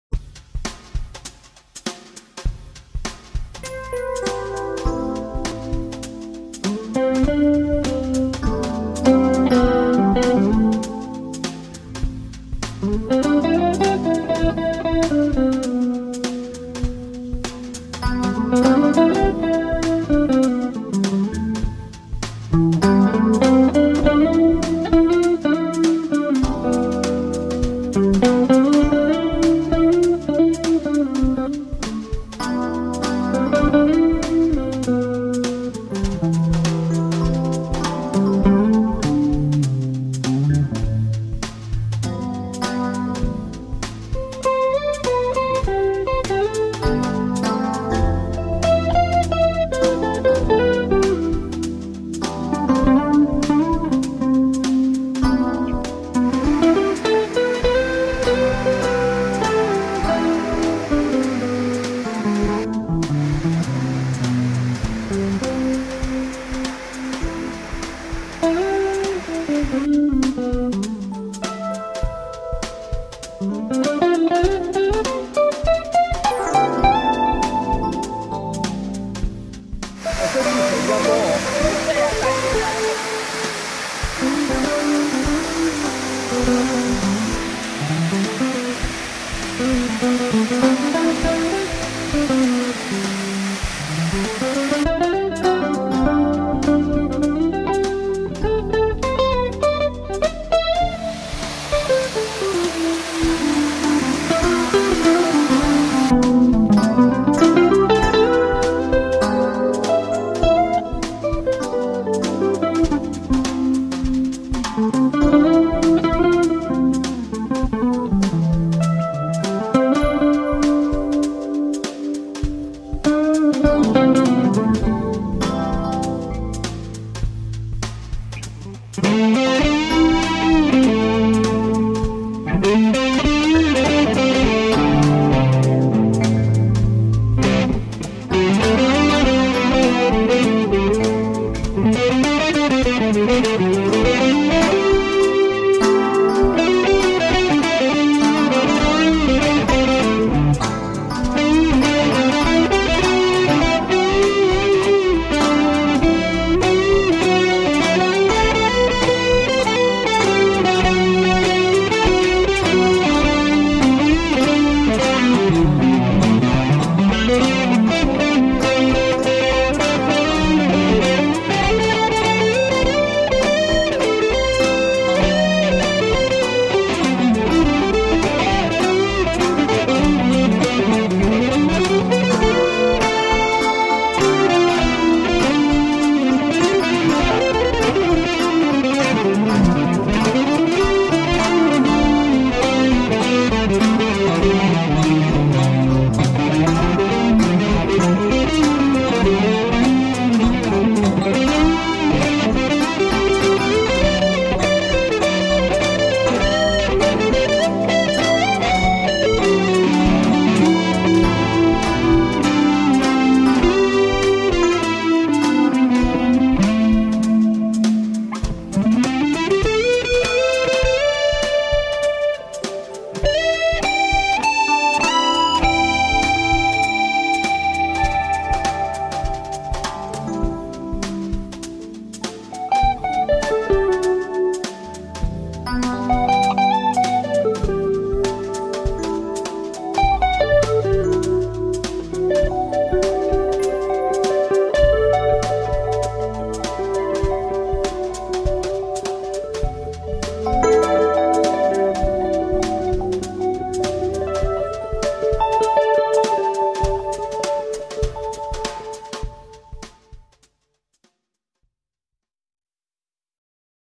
瓦拉米步道HD2015-12-21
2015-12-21我們走訪瓦拉米步道，這是我們第21次進入瓦拉米步道。